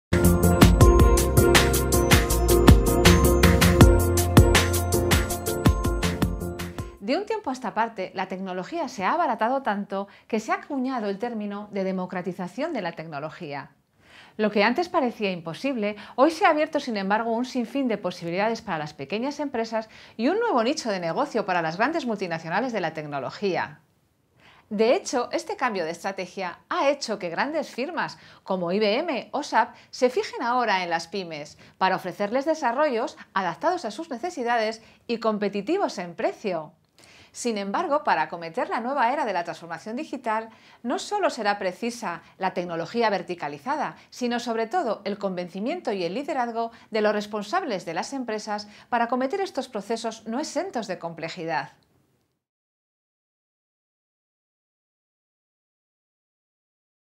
Luisa Alcalde: La democratización de la tecnología
De un tiempo a esta parte, la tecnología se ha abaratado tanto, que recientemente se ha acuñado el término de democratización de la tecnología.